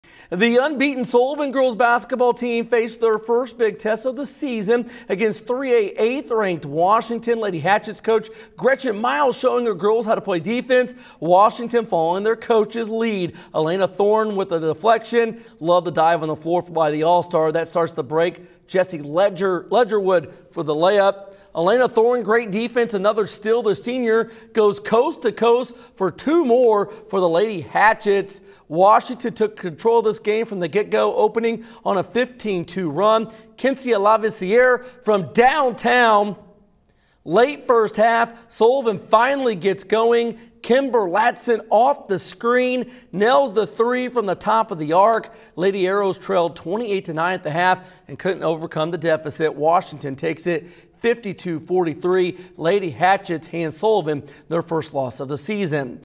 Washington Sullivan
Lady Hatchets hand Lady Arrows their first loss